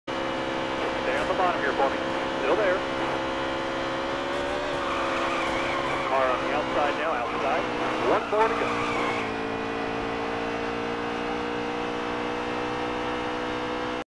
A Save that is Good
I can save a car on this terrible game cause I am good